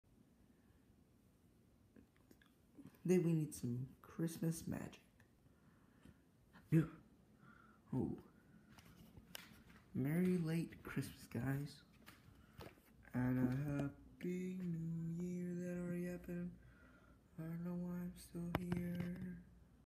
Merry Late Christmas Guys... (VERY LATE)
Hey guys! Watch as Punky prepares Christmas very late not knowing it's past December 25th... Lol, all joking aside, I know I already did 2 Happy New Year videos, but I never got to thank you. Punky thanks you for a great 2016!